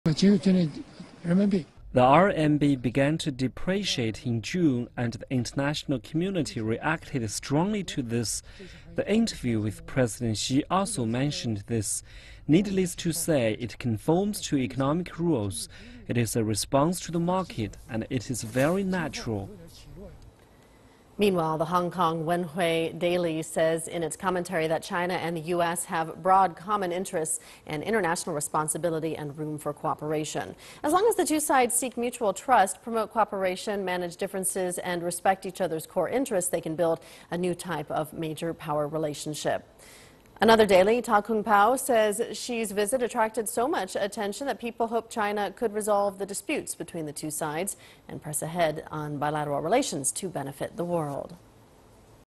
Expert: RMB depreciation is natural
Professor Yang Yunzhong, the former Executive Director of One Country Two Systems Research Center at Macao Polytechnic Institute, says he expects President Xi's visit to the US will promote common development and cooperation between the two sides. He also says the depreciation of RMB is natural.